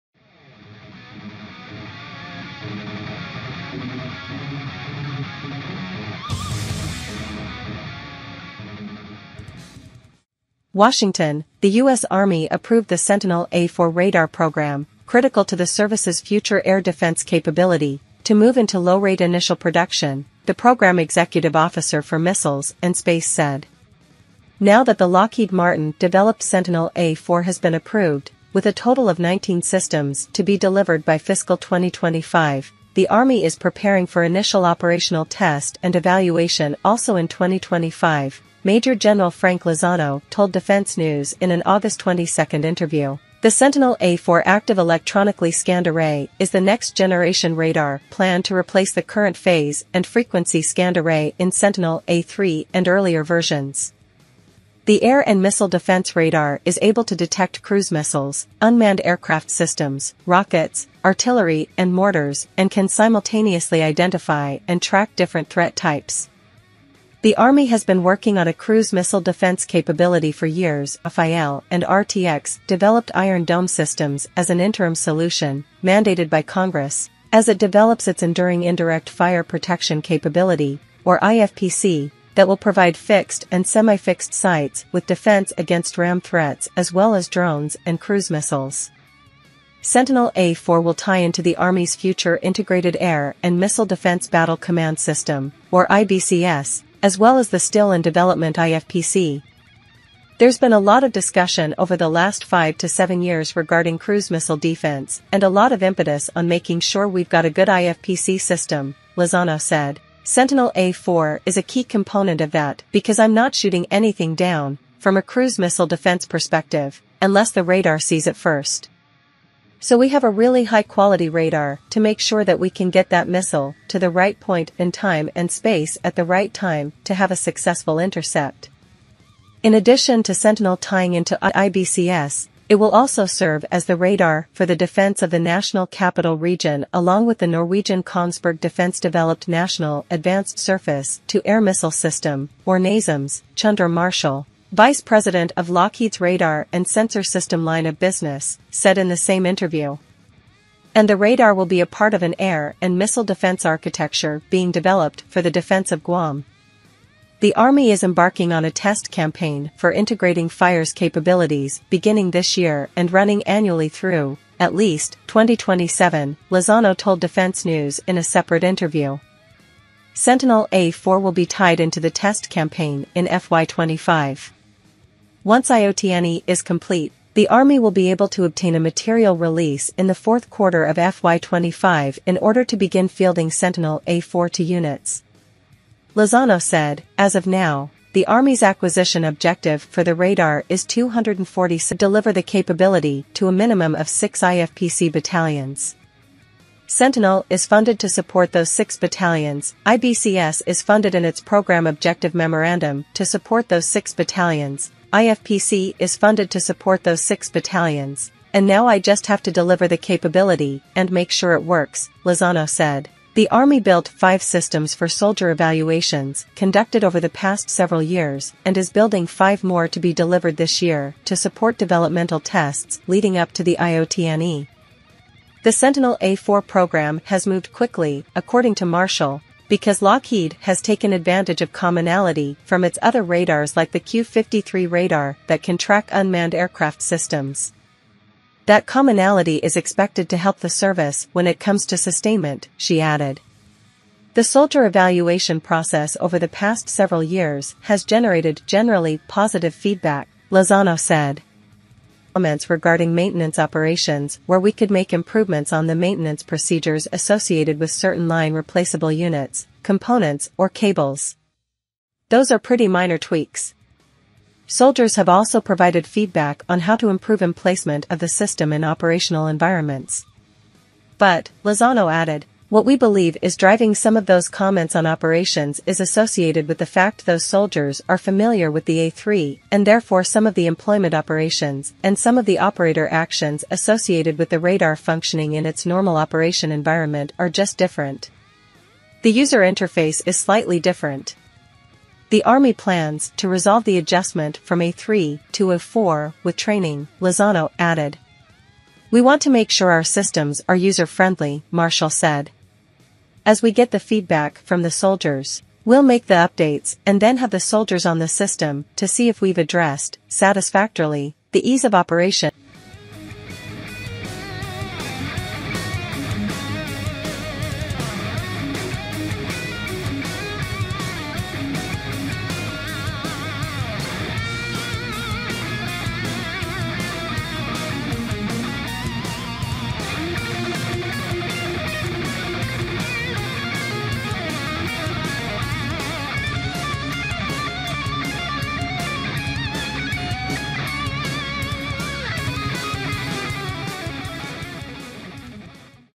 Good News: US Army approves Sentinel A4 air defense radar for low rate production
The U.S. Army approved the Sentinel A4 radar program, critical to the service’s future air defense capability, to move into low-rate initial production, the program executive officer for missiles and space said.